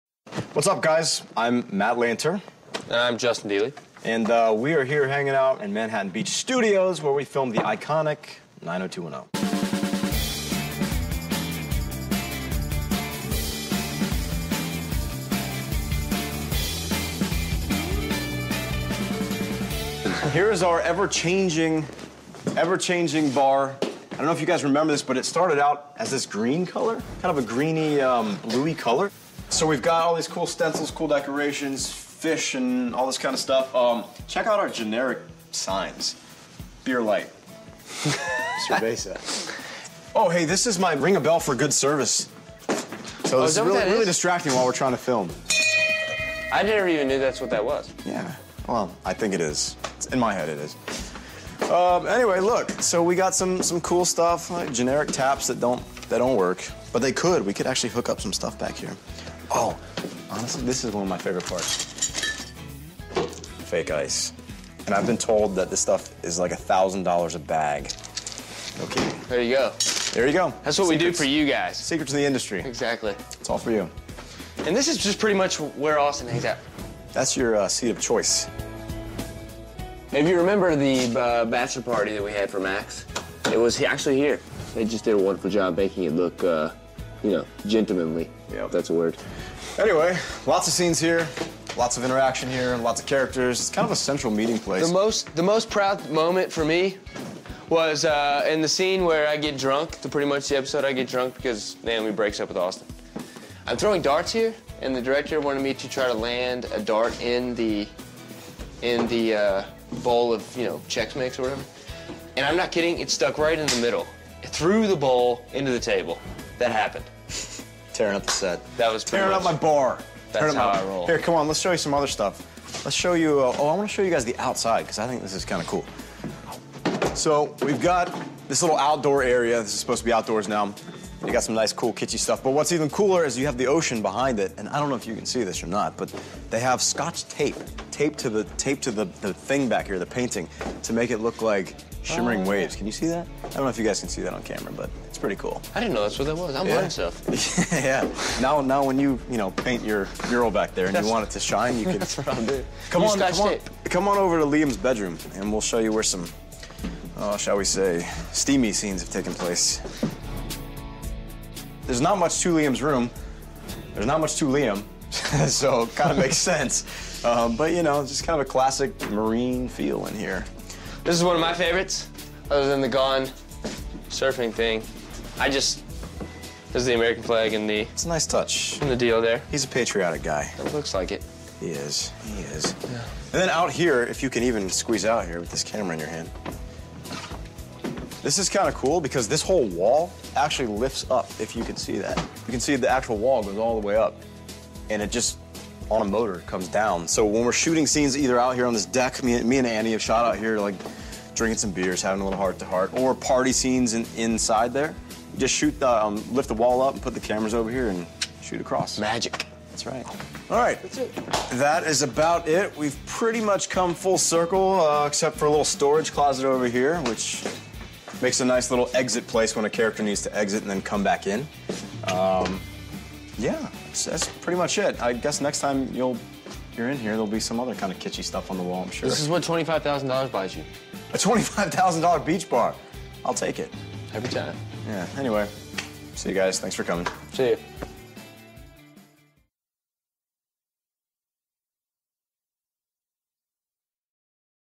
90210 Set Tour with Matt Lanter
Matt Lanter &  Justin Deeley give us a tour of the Offshore Set on 90210